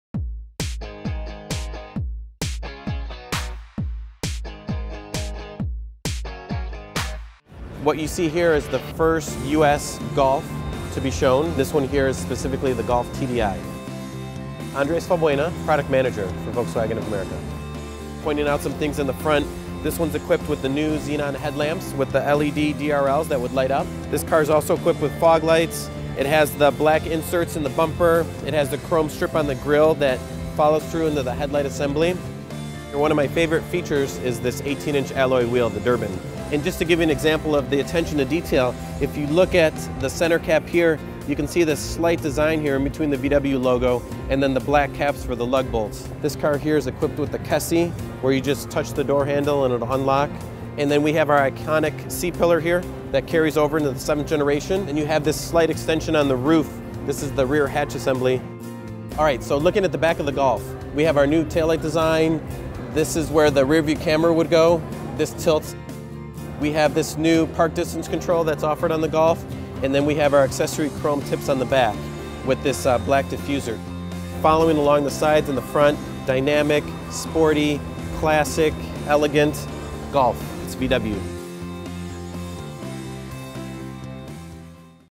VW Golf MK7 - A Hatchback for the Next Generation
The all-new Volkswagen Golf brings performance to a whole new level. Dynamic and sporty has been the hallmark of the Golf since its introduction in the  market as the Golf TDI.  Now, with 18-inch alloy wheels, LED fog lights and Bi-xenon headlights.

Displayed at the North American International Auto Show, the all-new Golf MK7 is a vehicle for enthusiasts, a car that combines performance and style down to the accents on its grill.  From windows to gauges, headlights to front angles, the MK7 enhances the Golf. Lending style to performance, the MK7 is a new standard for the Golf.